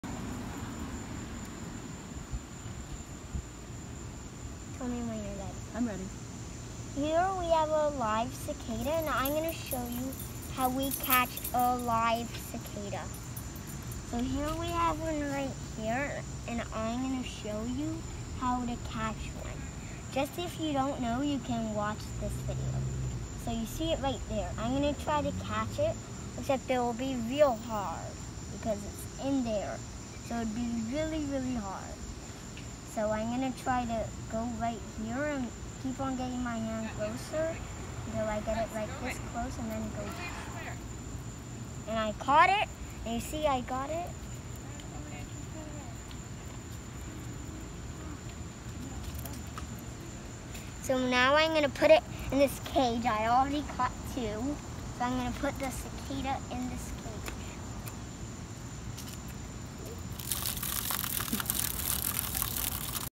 How I catch cicadas
This video shows how I catch cicadas in my neighborhood. I only keep them for a few hours and then release them back outside. I hope this video will help you learn to catch cicadas!